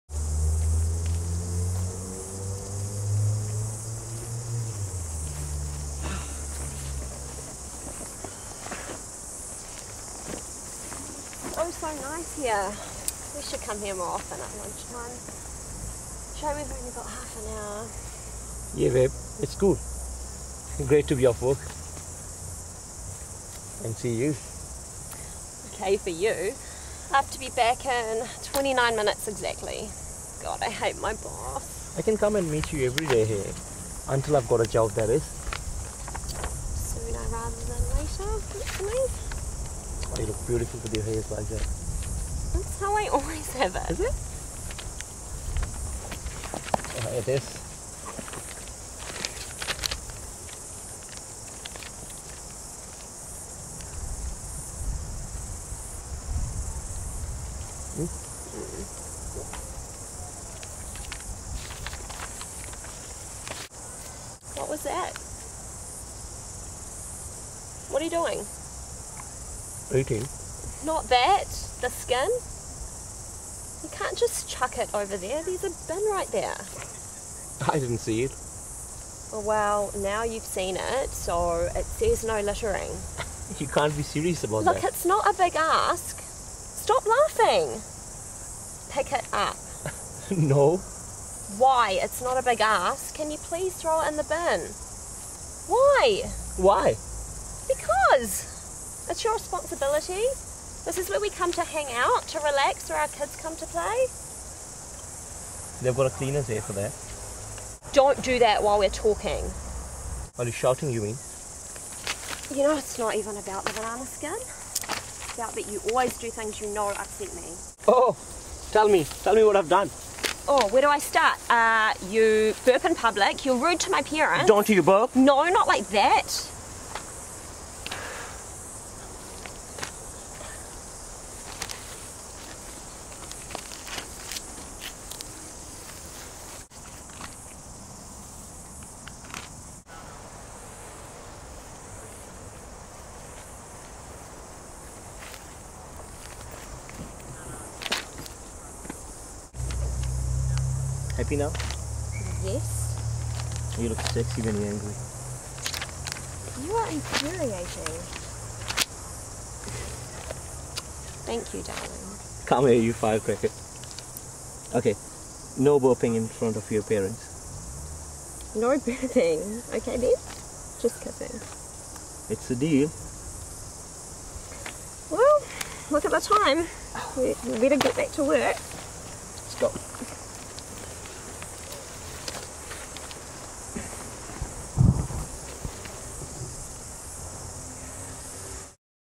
Studio 111 Screen Acting School Auckland New Zealand
A Studio 111 Screen Acting Scene on location as part of the Level1 10 week course 2013. Filmed by Paulus Romijn from the Presenters Platform .